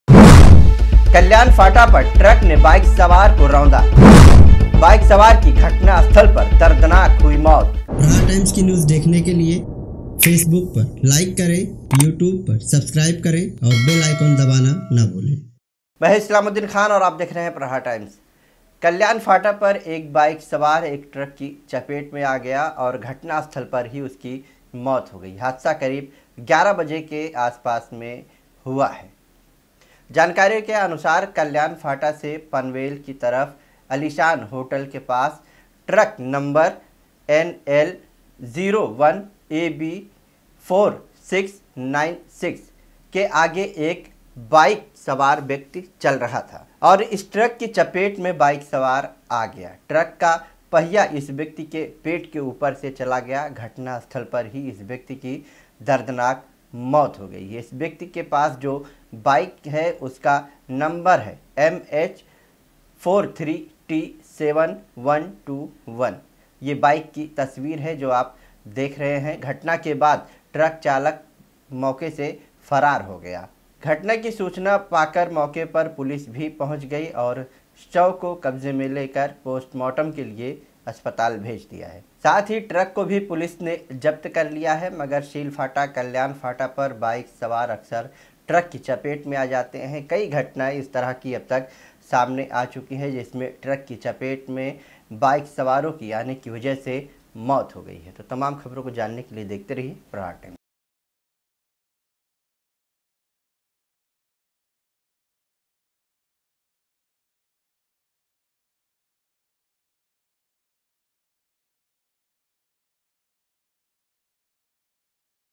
ट्रक के नीचे आया बाइक सवार घटना स्थल पर मौत || Islamuddin khan [BREAKING NEWS] ||Aapka Prahar Times
ट्रक के नीचे आया बाइक सवार घटना स्थल पर मौत ||Islamuddin khan[BREAKING NEWS]

कल्याण फाटा पर ट्रक के नीचे आने से एक बाइक सवार की घटना स्थल पर ही मौत हो गई घटना आज 11 बजे के आसपास में हुई है